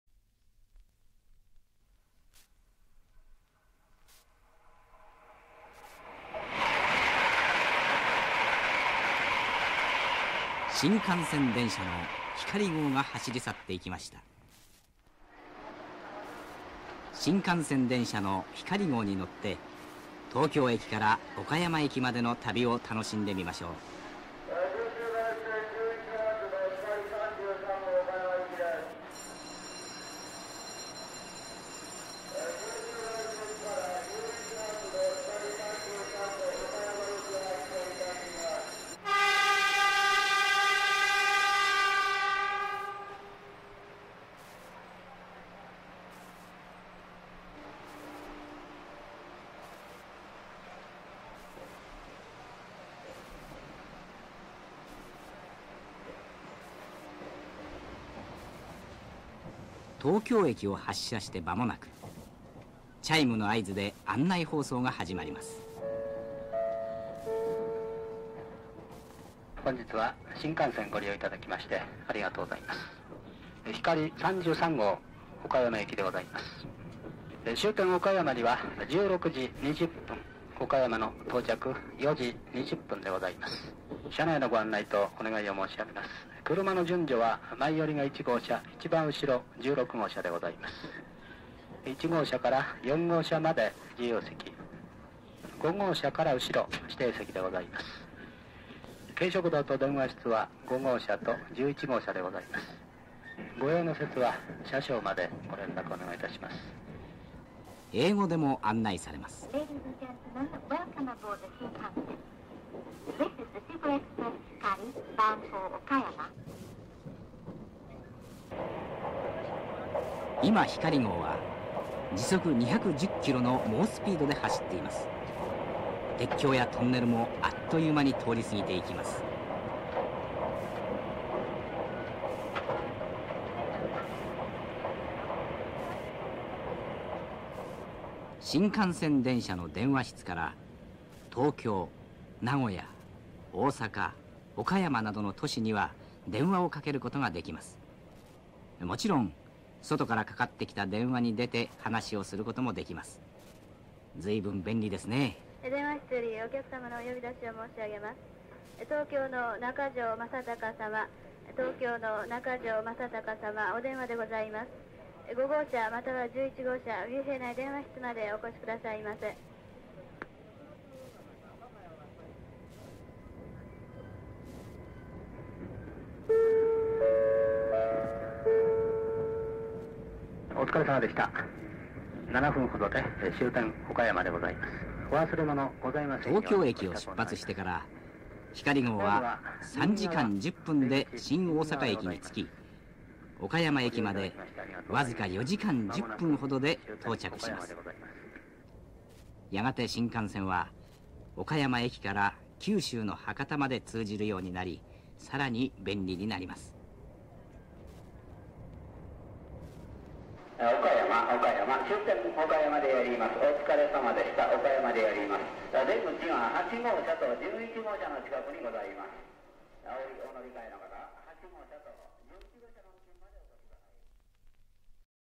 新幹線「ひかり号」（昭和５０年以前）
岡山駅が終点の頃，昭和４７年から昭和５０年頃の音源です。